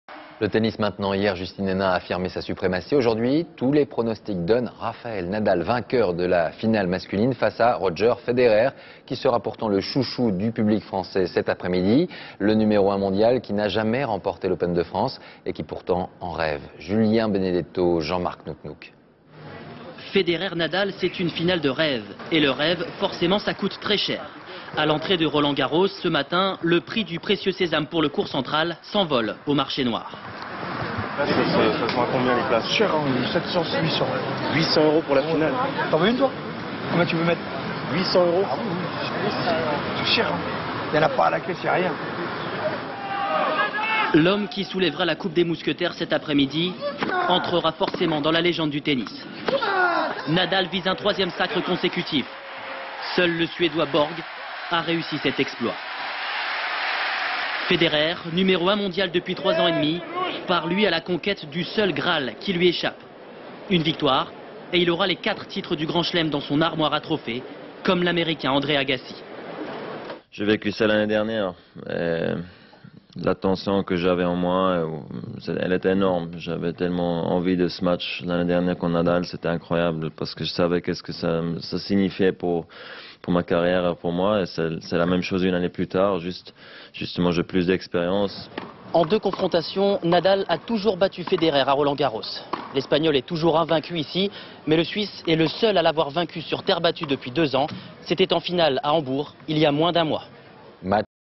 Rafael Nadal, favori à Roland Garros | Archive INA
10 juin 2007
 [Source : prompteur France 2]
     Le tennis maintenant ...
     Hier, Justine Henin a affirmé sa suprématie...
     Aujourd'hui, tous les pronostics donnent Rafael Nadal vainqueur de la finale masculine... face à Roger Federer qui sera le chouchou du public français cet après midi     Le numéro un Mondial qui n' a jamais remporté l'Open de France et qui en rêve pourtant...
     Julien BENEDETTO, Jean-marc NOUCK-NOUCK Images d'archive INA
Institut National de l'Audiovisuel